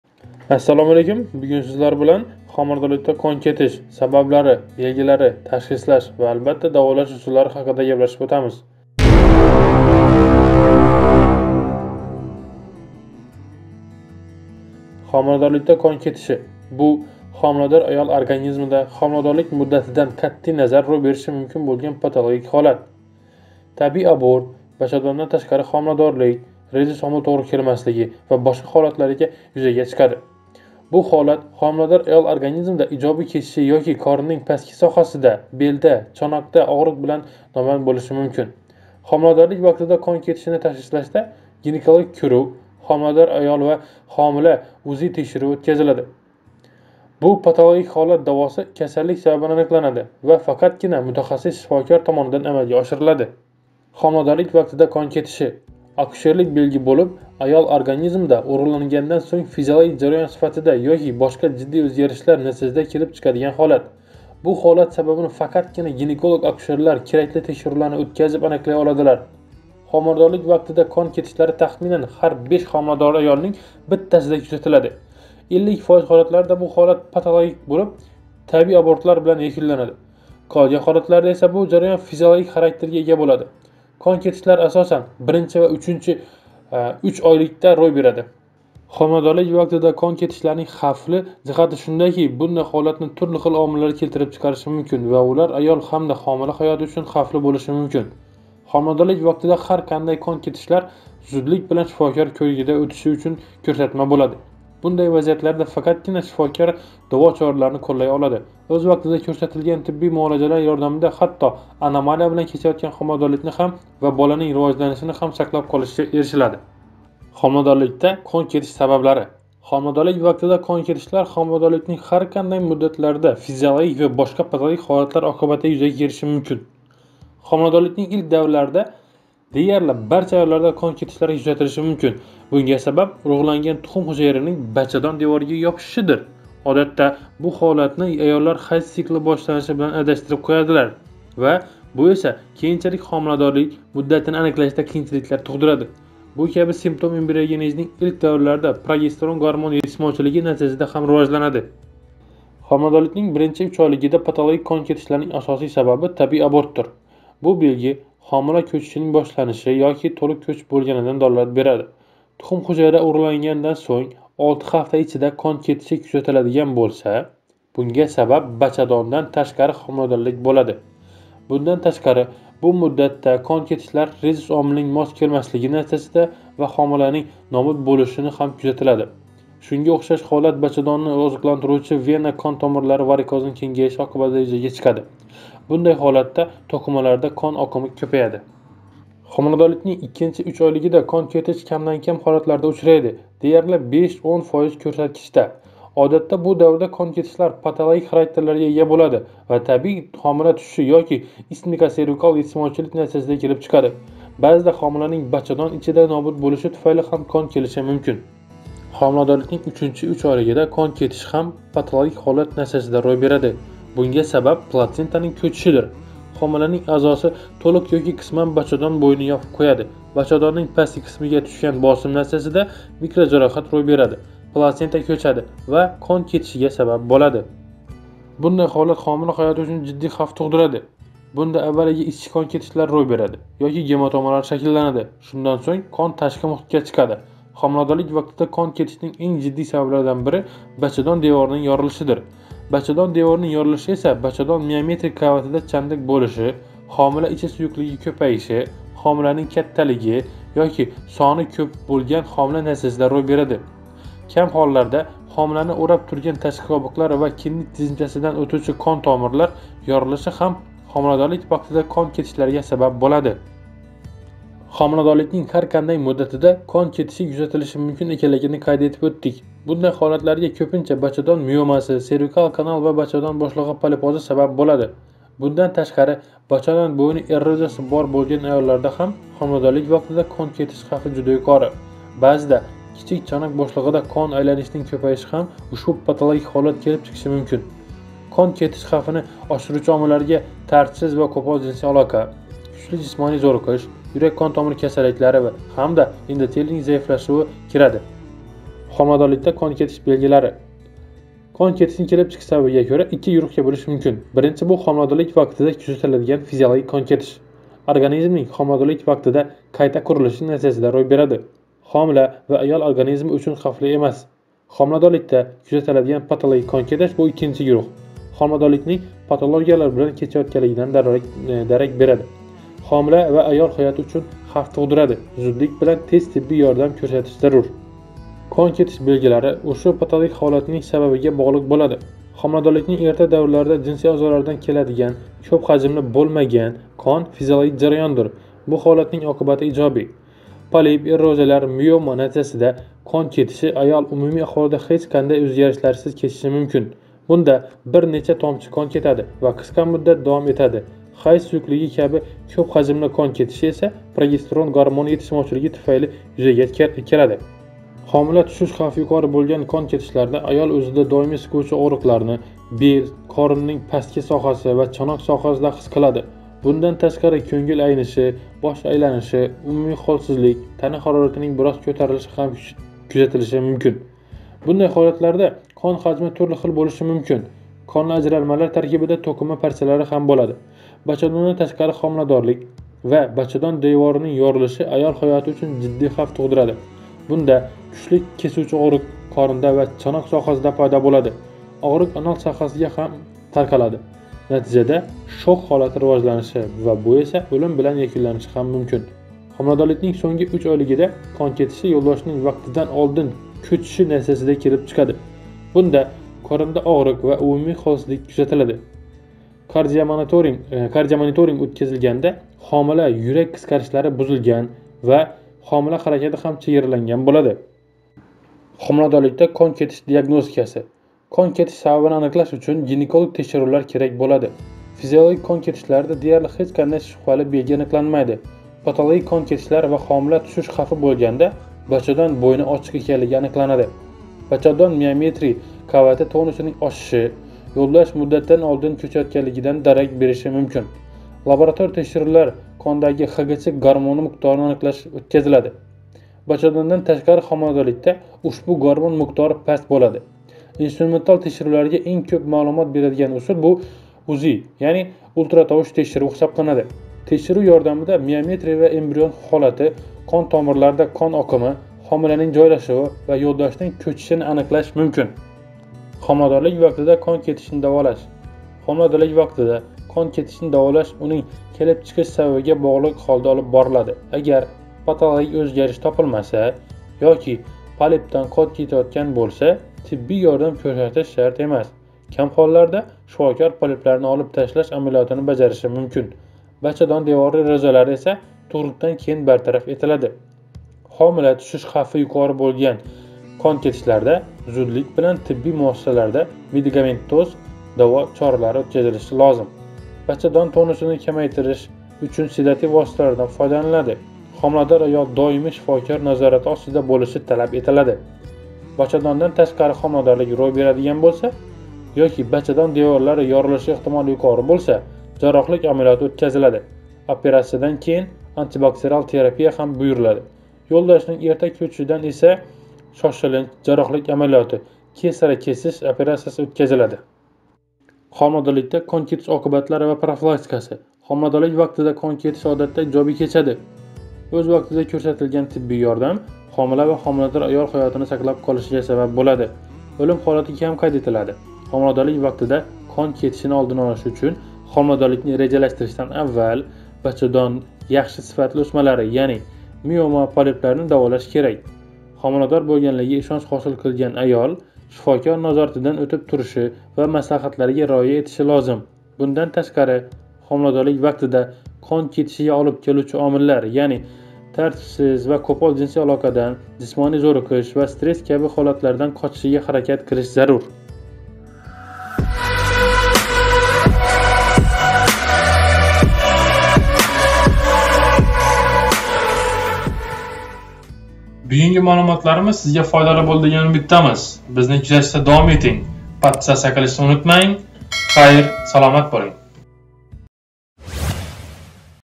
HOMILADORLIKDA QON KETISHI – SABABLARI, BELGILARI, TASHXISLASH VA DAVOLASH USULLARI
Homiladorlikda qon ketishi – homilador ayol organizmida homiladorlik muddatidan qatʼiy nazar roʻy berishi mumkin boʻlgan patologik holat. Tabiiy abort, bachadondan tashqari homiladorlik, rezus omil toʻgʻri kelmasligi va boshqa holatlarda yuzaga chiqadi. Bu holat homilador ayol organizmida ijobiy kechishi yoki qorinning pastki sohasida, belda, chanoqda ogʻriq bilan namoyon boʻlishi mumkin.